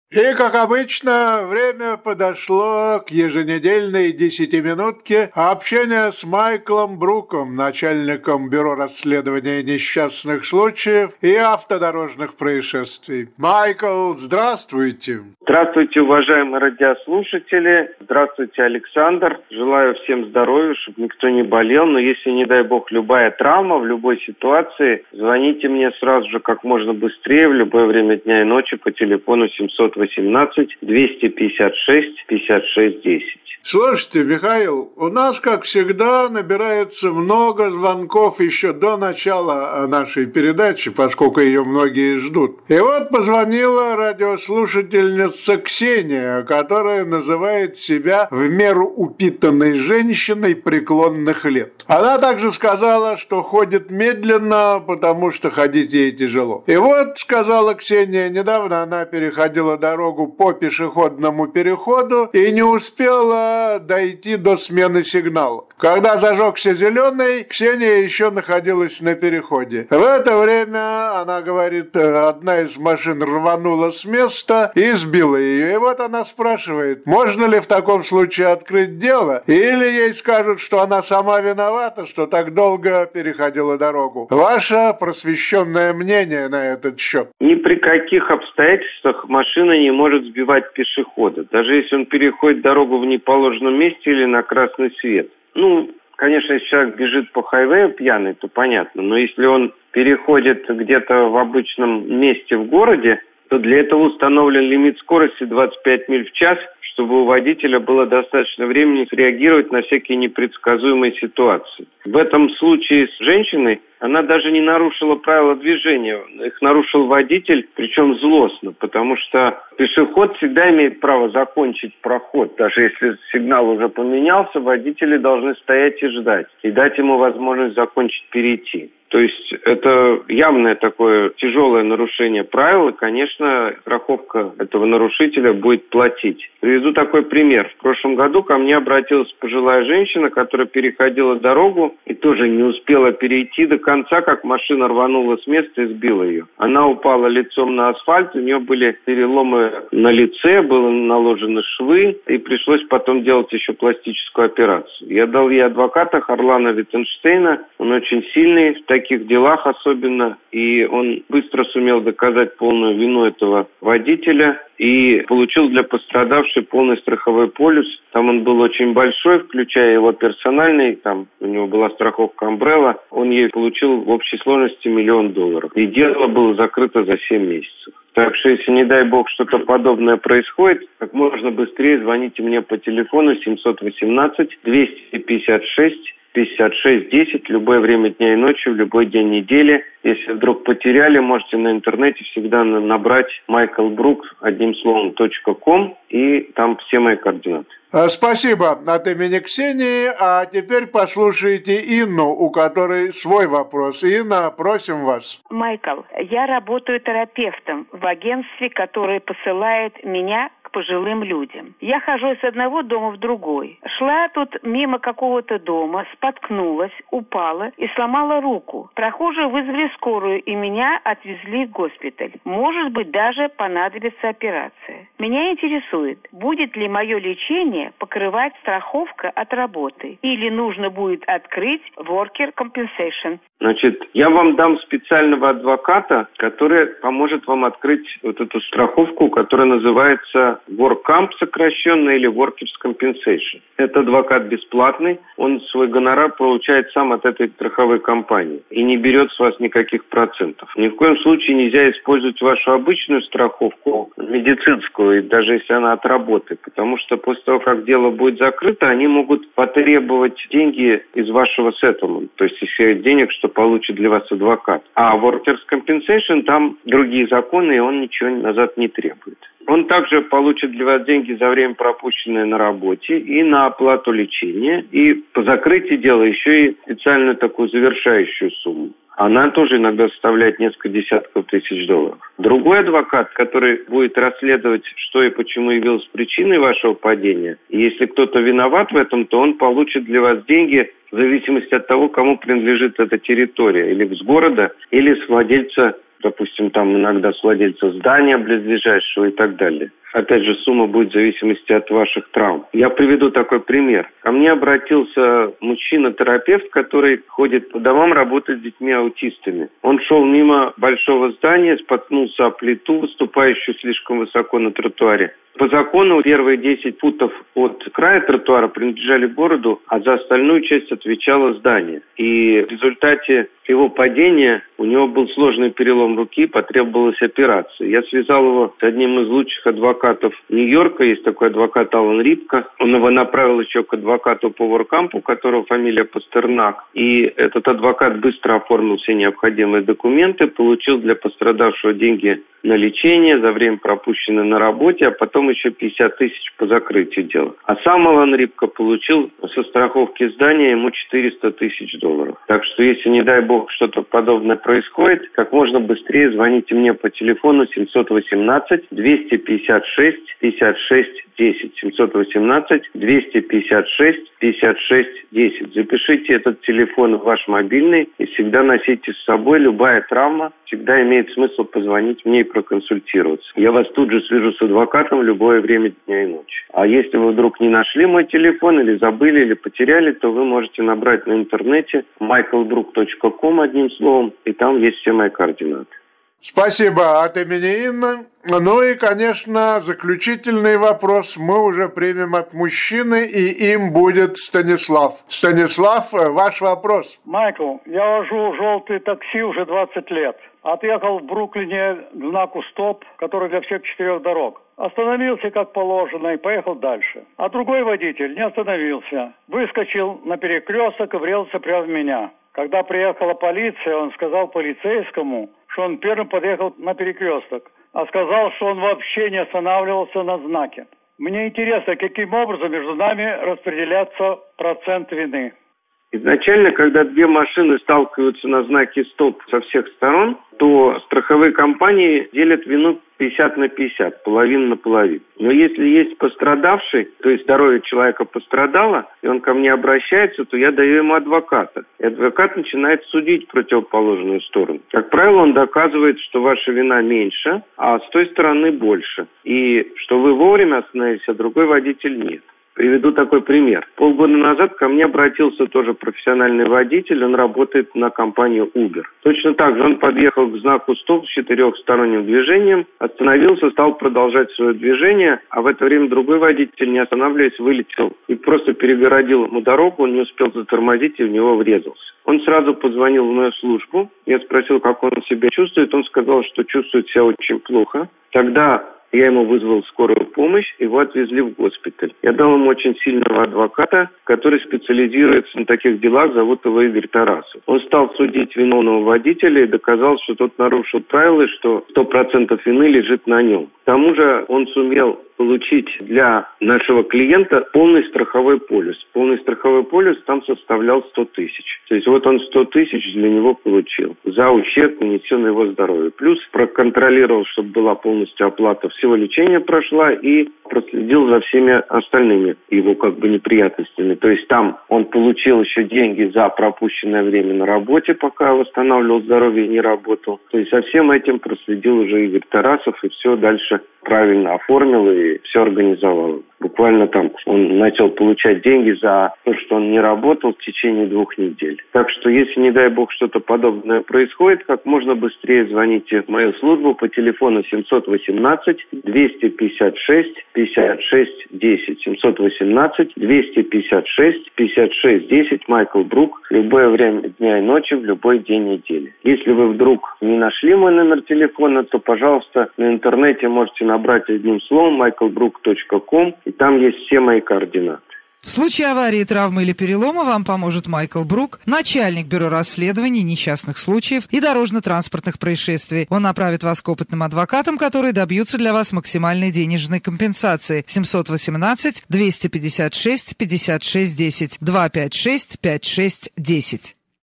Радио запись эфира [08.10.2021] MichaelBrook.com
Майкл Брук / Michael Brook

Нью-Йорк, Нью-Джерси / New-York, New-Jersey

В радио передачах рассмотрены лишь некоторые из случаев получения травм на строительстве, в автомобильных авариях, травмы и переломы полученные при падениях и других несчастных случаях. Чтобы узнать больше о размере компенсации и тех мерах которые необходимо предпринимать в подобных ситуациях, свяжитесь с нами по номеру телефона или заполните форму обратной связи посетив наш сайт